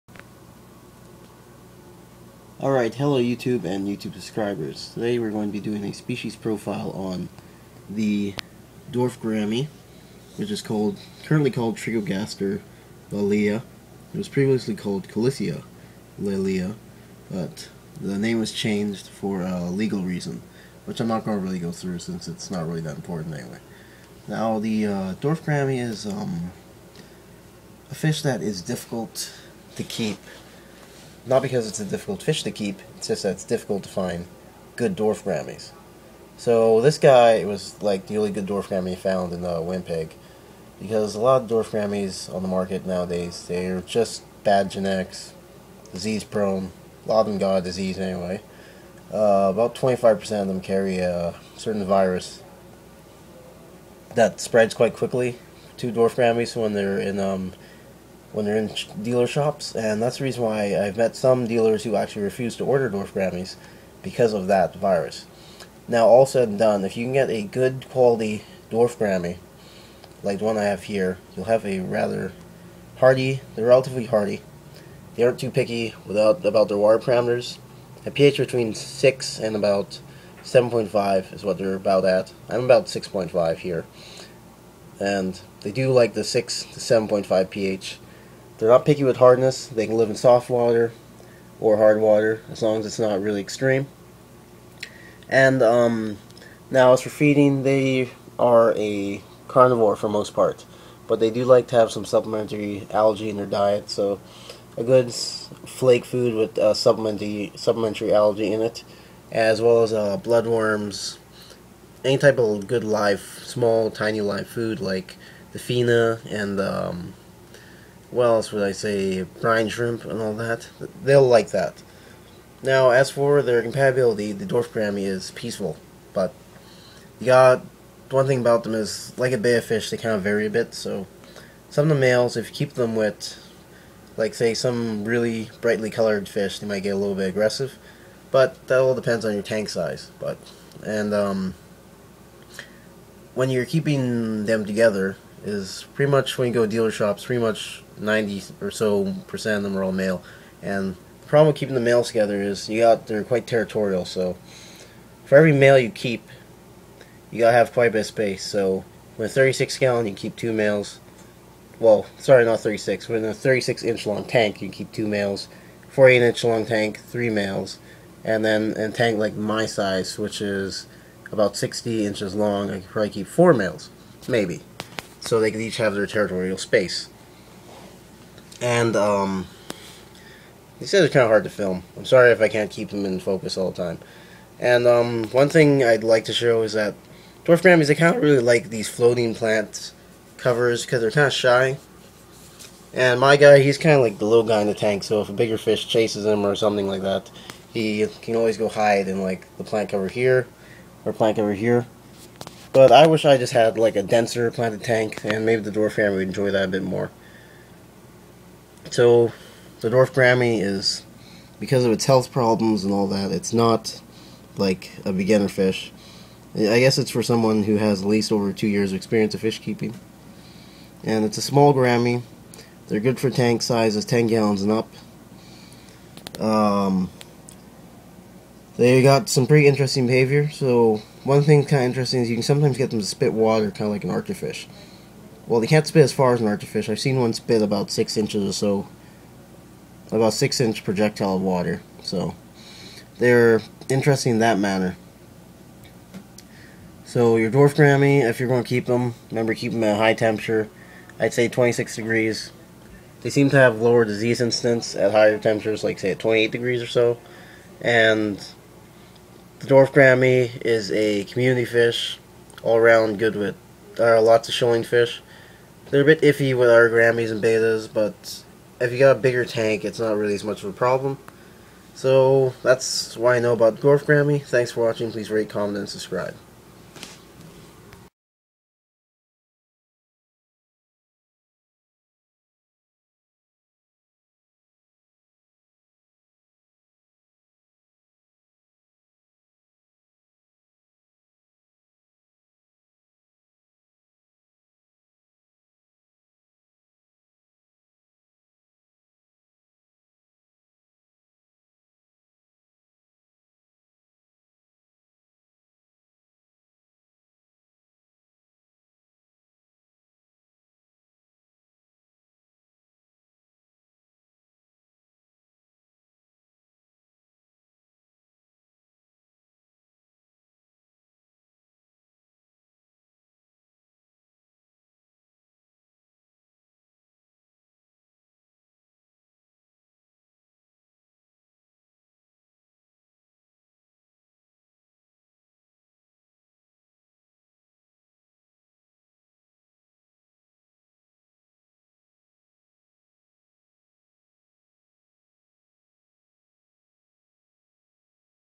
Species Profile # 4: The Dwarf Gourami (Trichogaster lalius)
A profile on the dwarf gourami to aid rookie fish keepers